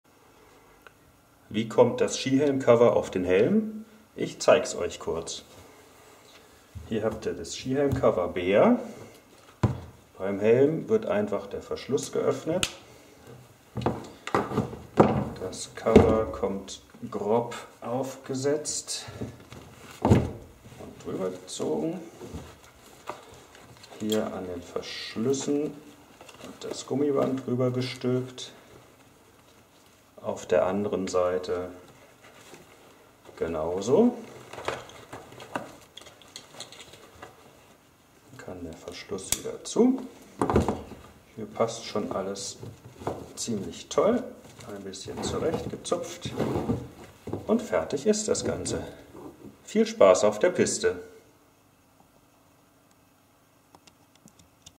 28-5009 grauer Bär Ski Helm Cover Video - Fetenman via party-deko-shop.de
Ein kurzes Erklär-Video vom party-deko-shop.de, das zeigt, wie das Skihelmcover auf einen Skihelm aufgezogen wird.

Der Fetenman macht auch Deine Party bunt!